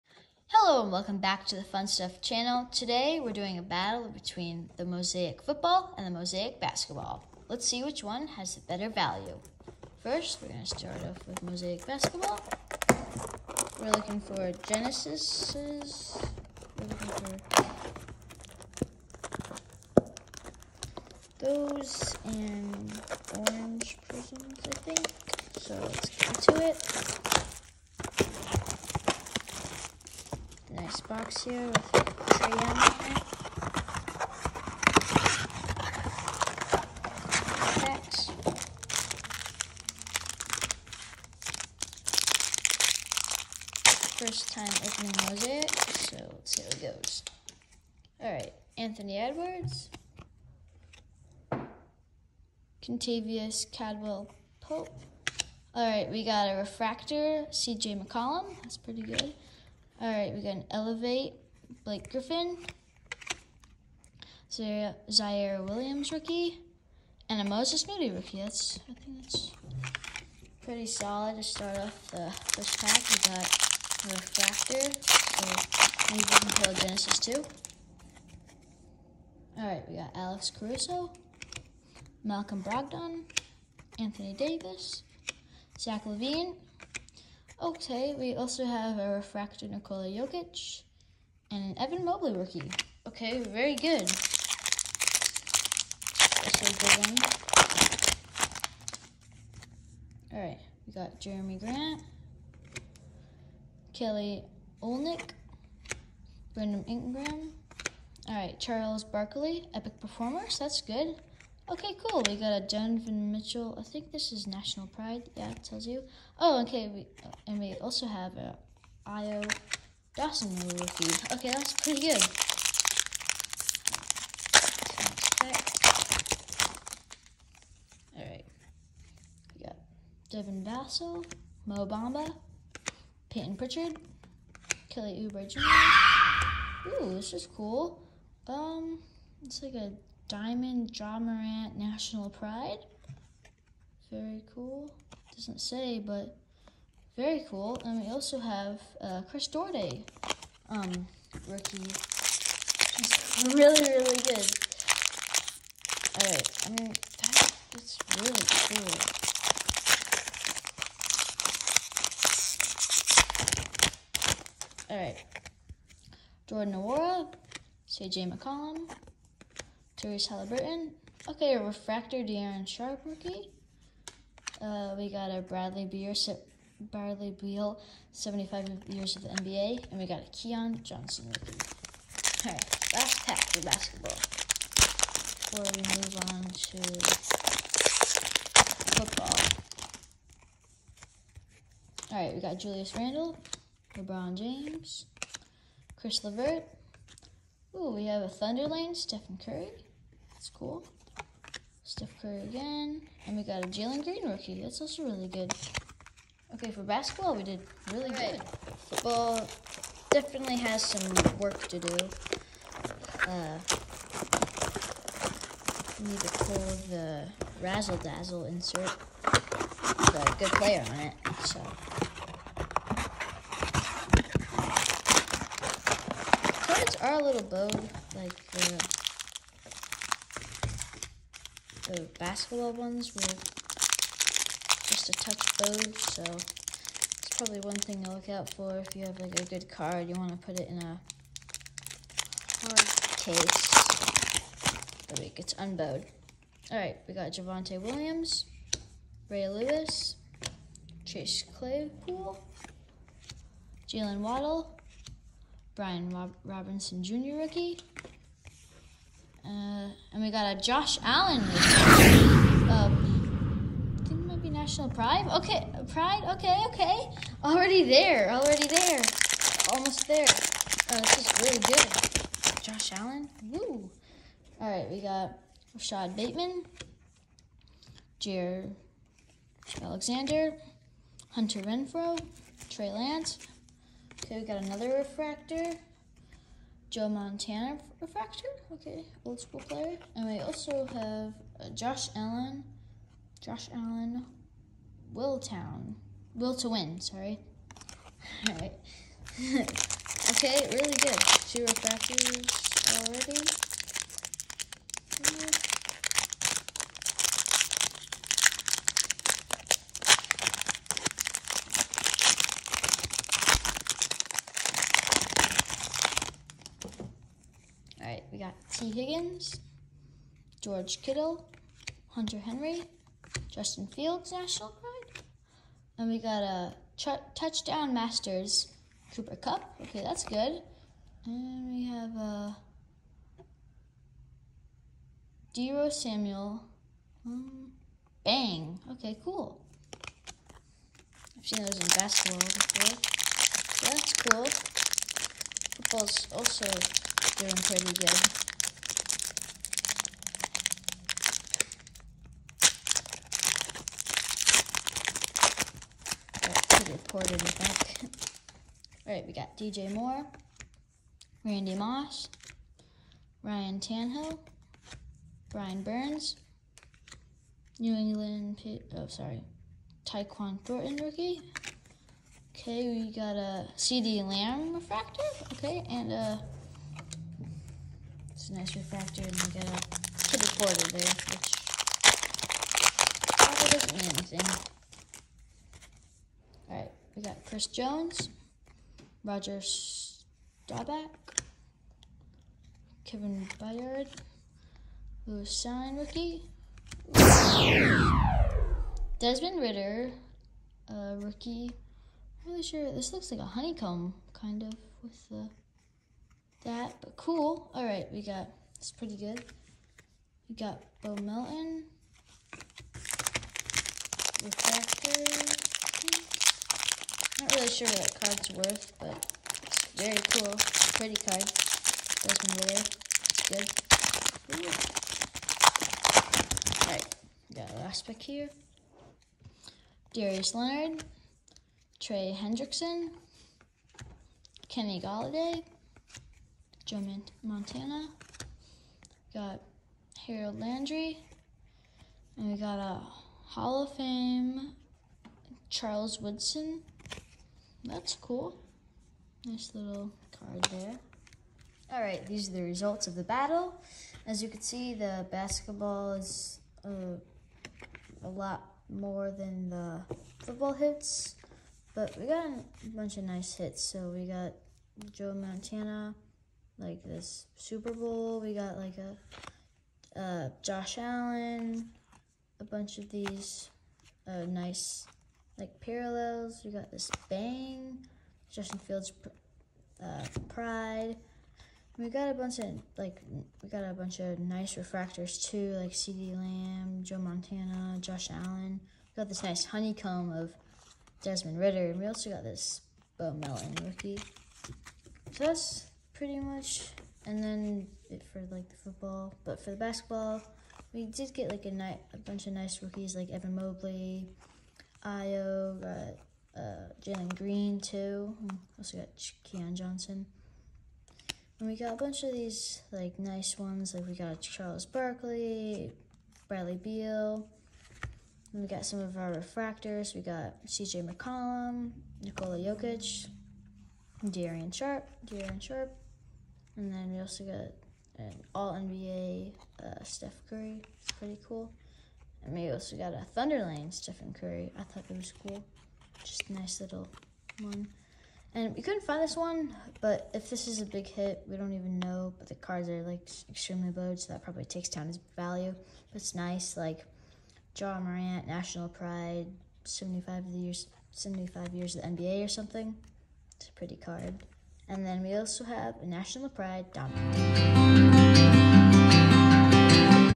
Did We Pull Two Case Hits? Mosaic Basketball and Mosaic Football
The Fun Stuffers rip a Mosaic Basketball blaster and a Mosaic Football blaster. The Fun Stuffers go on a bit of a heater with two case hits.